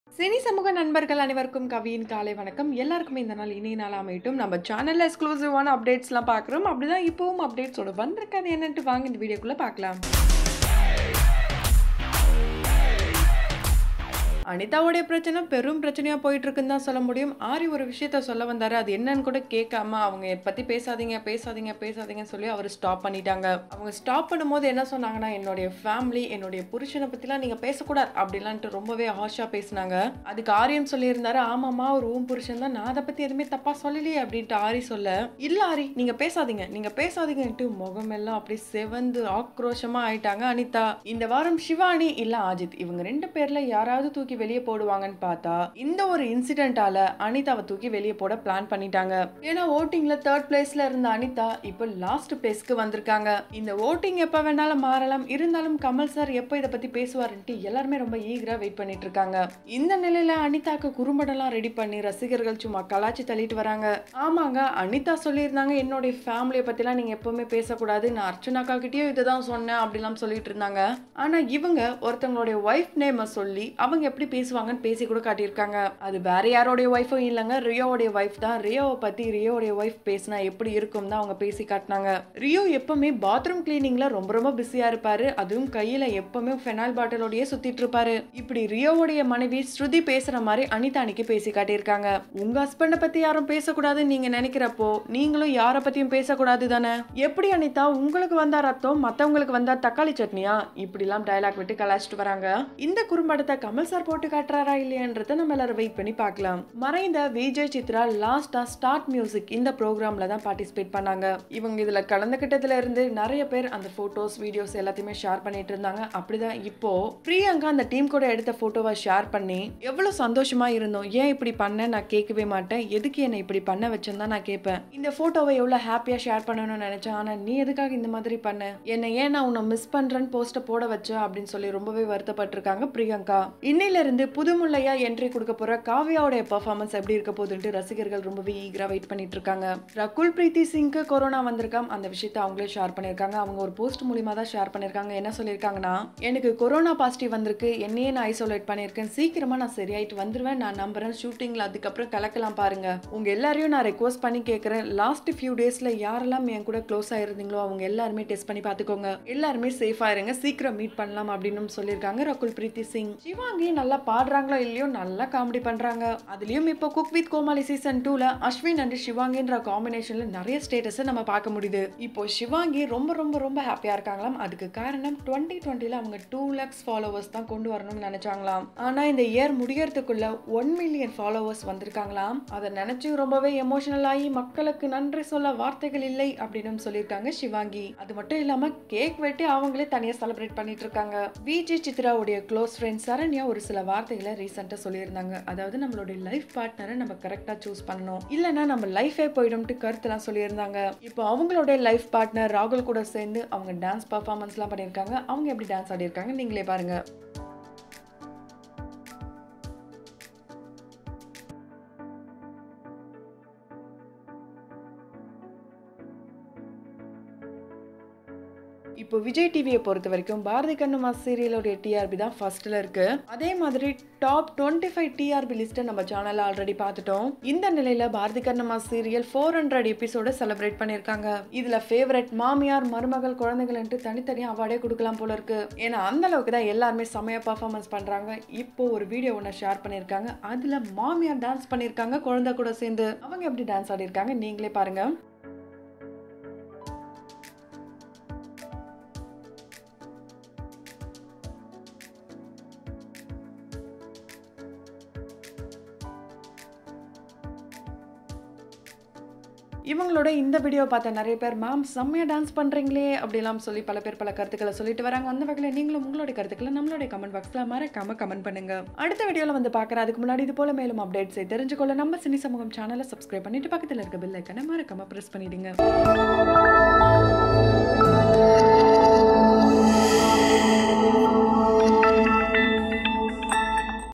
Anitha's kurrumpadam goes on viral || Singer Shivangi latest || Start Music VJ Priyanka
-Bigg Boss fans made kurrumpadam for Anitha, that she has spoken about rio and his wife sruthi. But in yesterday's show, she fights with aari.
-VJ Priyanka emotional post about VJ Chithra and her last show in Start Music
-Cook with Comali fame & Singer Shivangi got 1Million followers on instagram
-Rakul Preet Singh message their fans and friends
-Serial Actress Sharanya latest dance video
-Bharathi Kannamma serial shooting spot atrocities